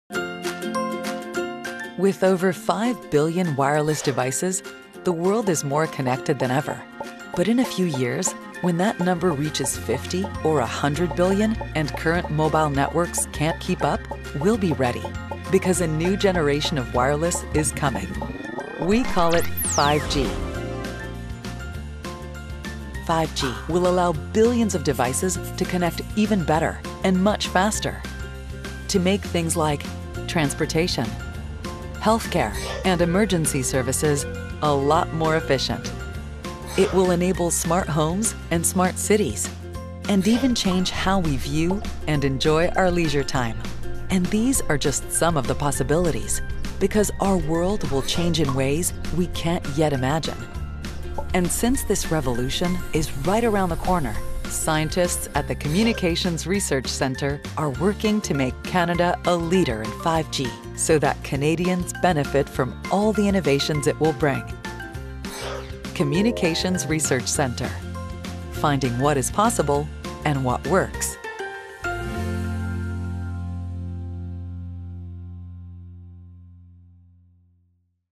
What is 5G?
Everyone is talking about 5G and the future of wireless. But what is it? Why is it important? This short animated video explores how 5G will change the world in ways we can't yet imagine and how Canada's Communications Research Centre is working to help Canada be a leader in 5G.